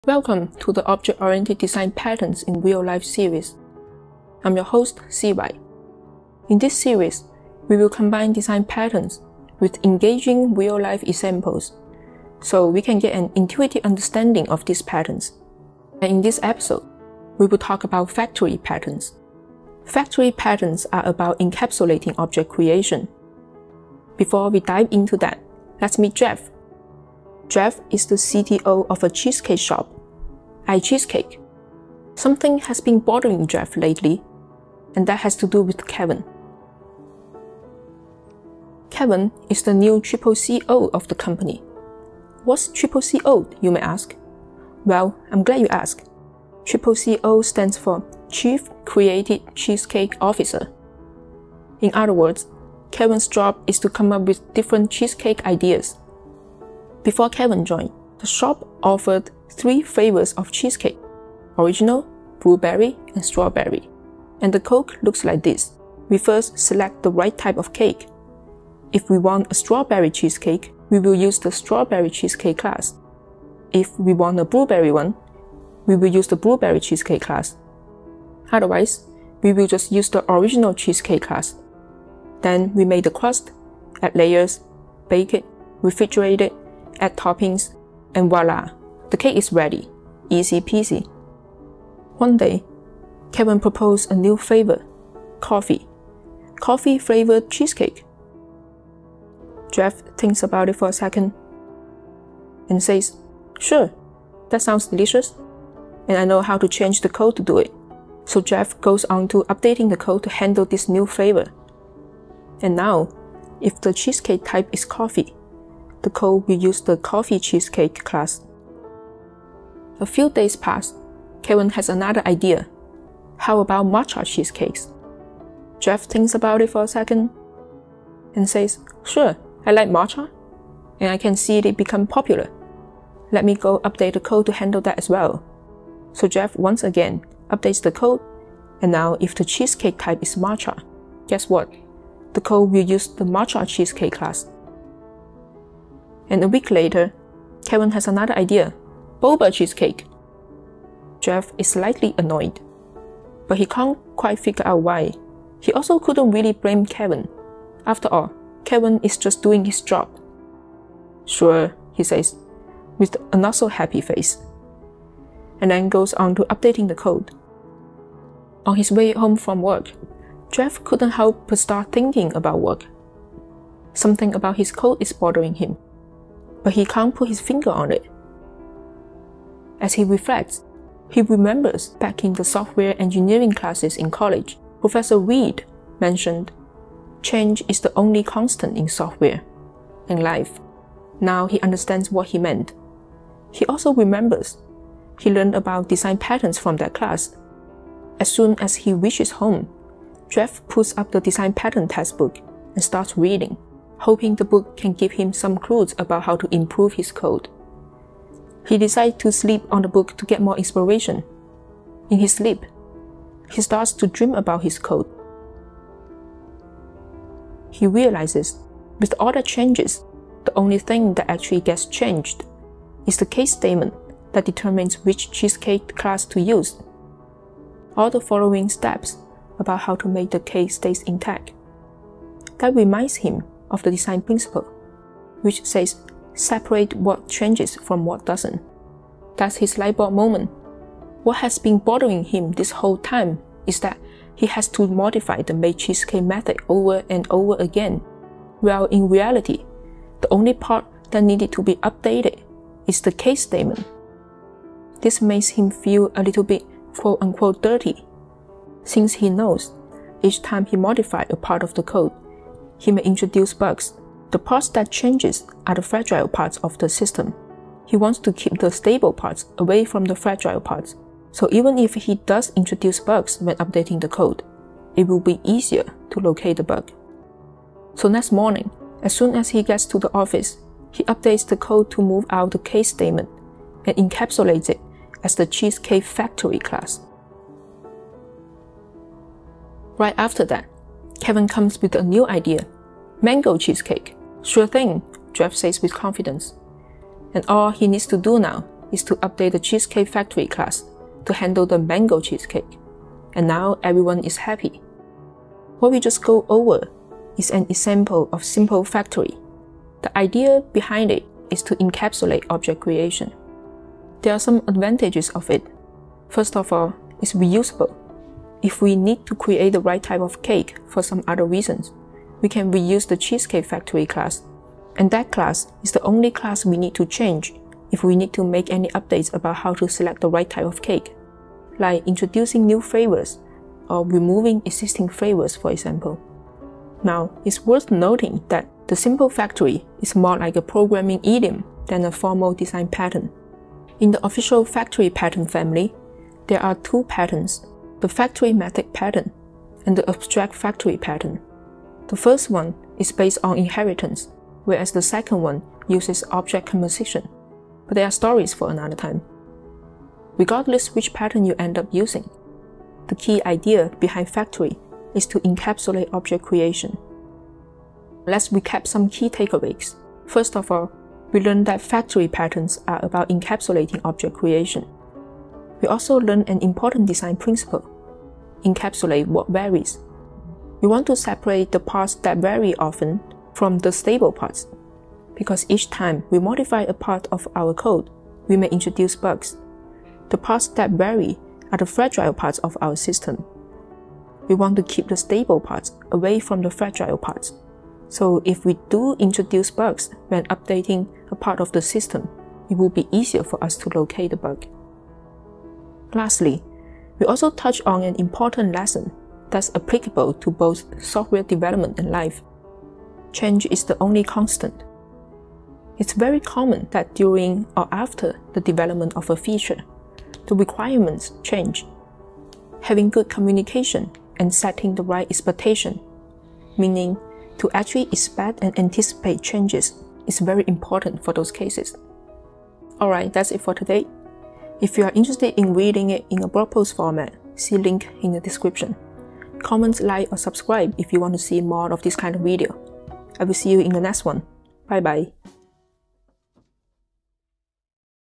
Simple Factory Design Pattern - In Real-Life
Welcome to the object-oriented design patterns in real life series.
In this series, we will combine design patterns with engaging real life examples, so we can get an intuitive understanding of these design patterns.
In this episode, we will talk about factory patterns, which are about encapsulating object creation.

---Notes

Simple factory is more like a programming idiom than a formal design pattern.
In the official factory pattern family, there are two pattern. The factory method pattern and the abstract factory pattern. The first one is based on inheritance where as the second one  leverages object composition. But they are stories for another time. Regardless which pattern you end up using, the key idea behind factory is to encapsulate object creation.

Takeaways:
1. Factory patterns are about encapsulating object creation.

2. Design principle: encapsulate what varies. We want to separate the parts that vary often from the stable parts. Because each time we modify a part of our code, we might introduce bugs. The parts that vary are the fragile parts of our system. We want to keep the stable parts away from the fragile parts, so if we did introduce bugs when updating a part of the system, it would be easier for us to locate the bug.

3. An important lesson that’s applicable to both software development and life : change is the only constant. It’s very common that during or after the development of a feature, the requirements change. Having good communication and setting the right expectation, meaning expecting and anticipating changes, is very important for those cases.

Topics: System Design, Software Engineering, Computer Science, Software Architecture, Refactoring